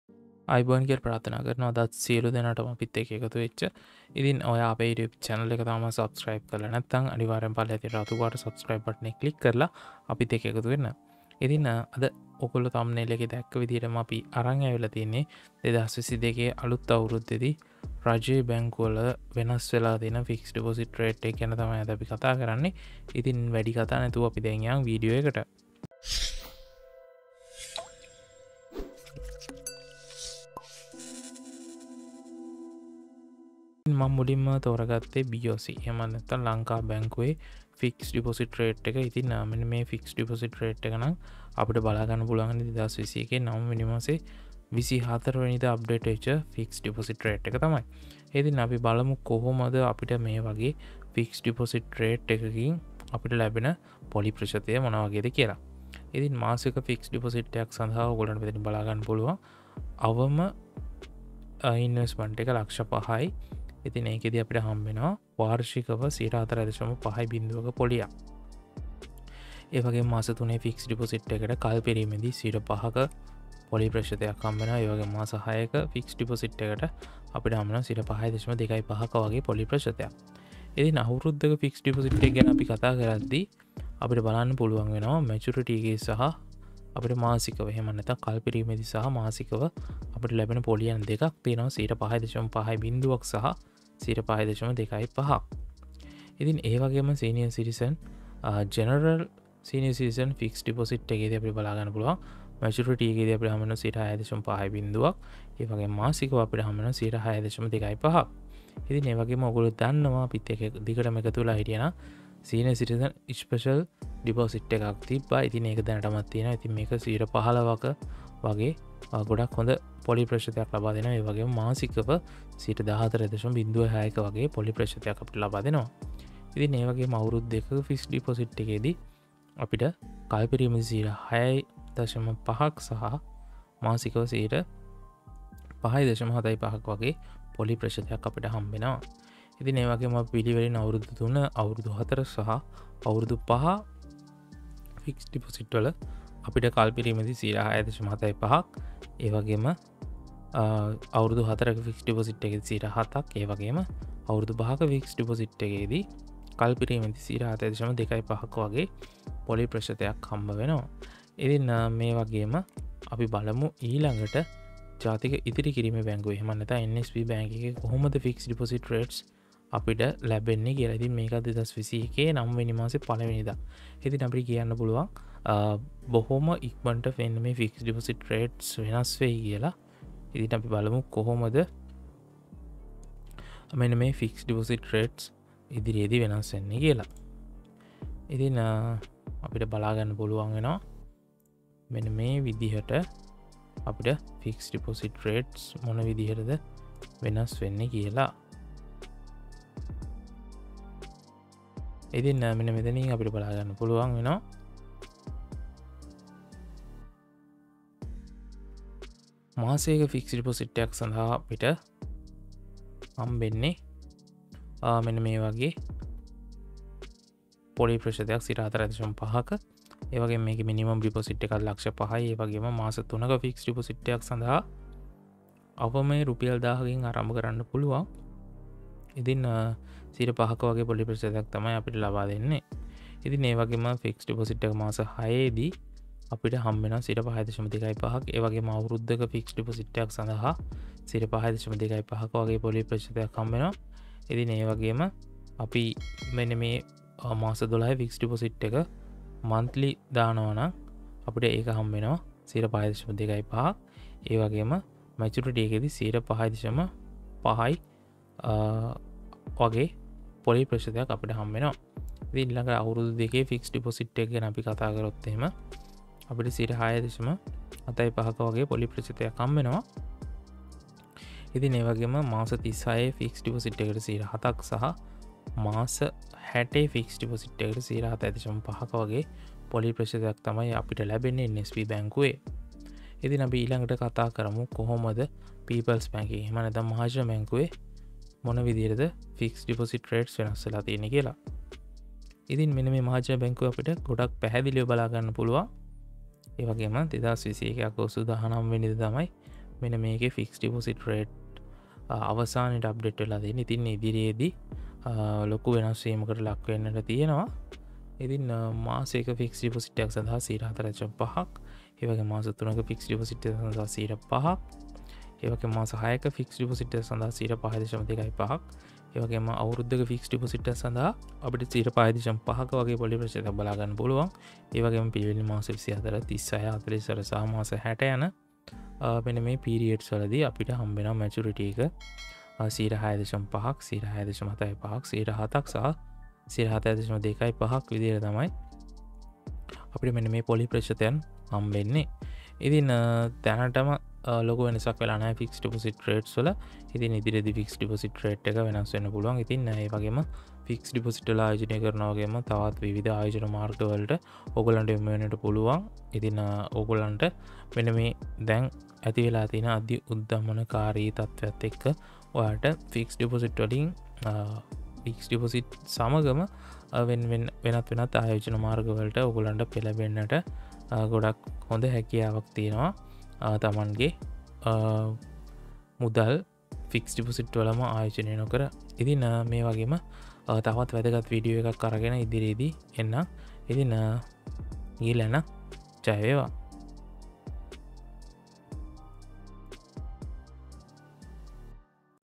New Fixed Deposit Rates Update in Sri Lankan Government Banks 2022
In this video, We are explaining about  New Fixed Deposit Rates Update in Sri Lankan Government Banks 2022

ඔබගේ ඕනෑම දැන්වීම්ක් ප්‍රචාරය සදහා අපව සම්බන්ද කරගන්න